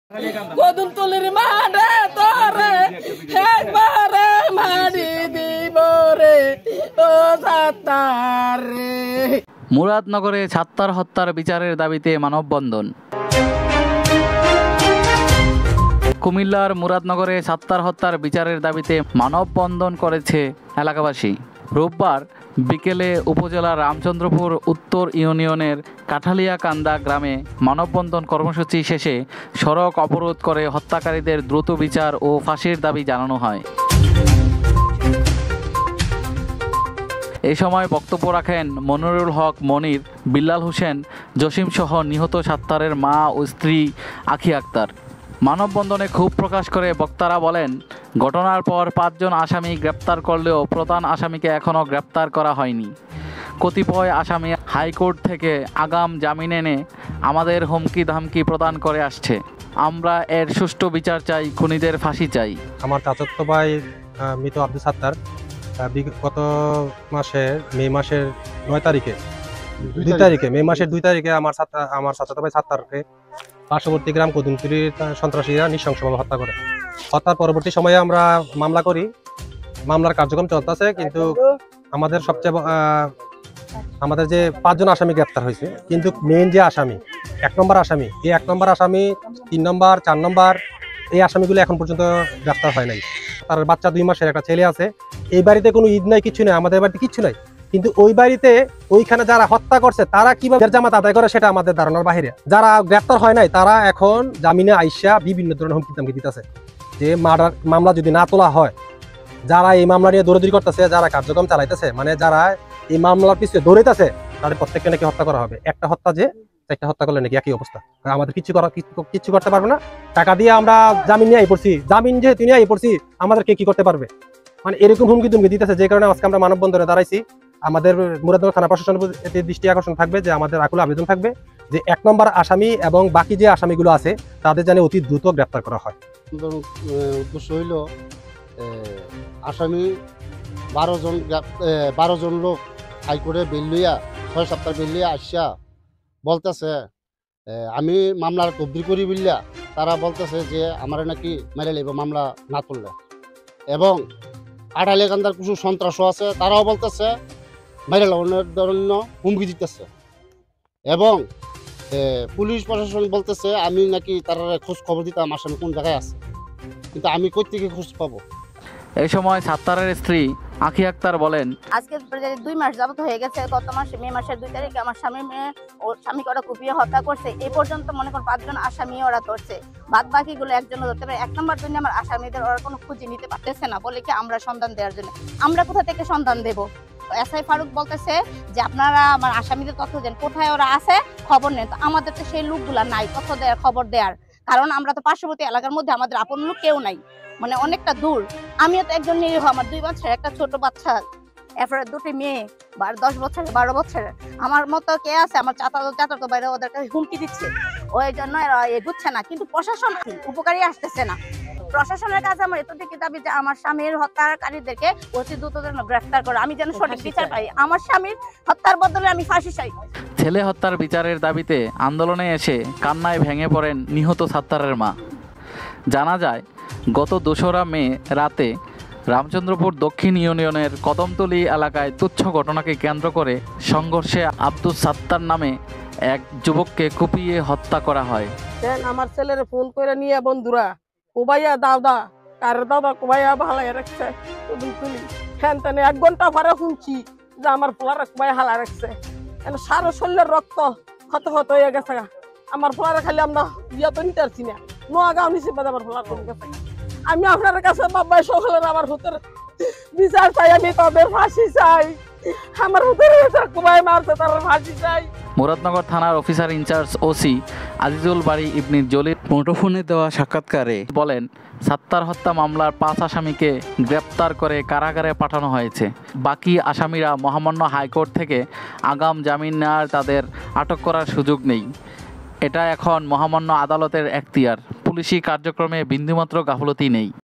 সাত্তার হত্যার বিচারের দাবীতে মানববন্ধন
this is murder case.
মুরাদনগরে সাত্তার হত্যার বিচারের দাবিতে মানববন্ধন

কুমিল্লার মুরাদনগরে সাত্তার হত্যার বিচারের দাবিতে মানববন্ধন করেছে এলাকাবাসী।

রোববার (০২ জুলাই) বিকেলে উপজেলার রামচন্দপুর উত্তর ইউনিয়নের কাঠালিয়াকান্দা গ্রামে মানববন্ধন কর্মসূচি শেষে সড়ক অবরোধ করে হত্যাকারীদের দ্রুত বিচার ও ফাসির দাবি জানানো হয়। এসময় বক্তব্য রাখেন, মনিরুল হক মনির, বিল্লাল হোসেন, জসিমসহ নিহত সাত্তারের মা ও স্ত্রী আখি আক্তার।

মানববন্ধনে ক্ষোভ প্রকাশ করে বক্তারা বলেন, ঘটনার পর ৫জন আসামী গ্রেপ্তার করলেও প্রধান আসামিকে এখনো গ্রেপ্তার করা হয়নি। কতিপয় আসামী হাইকোর্ট থেকে আগাম জামিন এনে আমাদের হুমকি ধামকি প্রদান করে আসছে। আমরা এর সুষ্ঠু বিচার চাই, খুনিদের ফাসি চাই।

এসময় সাত্তারের স্ত্রী আখি আক্তার বলেন, আজ ২মাস হয়ে গেলো আমার স্বামীর খুনিরা কয়েকজন গ্রেফতার হলেও বাকি আসামীরা এখনো আটক হয়নি। আমার ২মাসের একটা ছোট্ট বাচ্চা ও আরো দুটি মেয়ে নিয়ে অনেক কষ্টে দিনপার করছি। আমি আমার স্বামীর হত্যাকারীদের দ্রুত বিচারের আওতায় এনে দৃষ্টান্তমূলক শাস্তির দাবি জানাই।
ছেলে হত্যার বিচারের দাবিতে আন্দোলনে এসে কান্নায় ভেঙে পড়েন নিহত সাত্তারের মা।

জানাযায়, গত  ২রা মে রাতে রামচন্দ্রপুর দক্ষিন ইউনিয়নের কদমতলী এলাকায় তুচ্ছ ঘটনাকে কেন্দ্র করে সংঘর্ষে আবদুস সাত্তার (৩৫) নামে এক যুবককে কুপিয়ে হত্যা করা হয়।
মুরাদনগর থানার অফিসার ইনচার্জ (ওসি) আজিজুল বারী ইবনে জলিল বলেন সাত্তার হত্যা মামলার ৫ আসামীকে গ্রেফতার করে কারাগারে পাঠানো হয়েছে। বাকি আসামিরা মহামান্য হাইকোর্ট থেকে আগাম জামিন নেয়ায় তাদের আটক করার সুযোগ নেই। এটা এখন মহামান্য আদালতের এখতিয়ার। পুলিশী কার্যক্রমে বিন্দুমাত্র গাফিলতি নেই।

@মৃত্যু
@খুন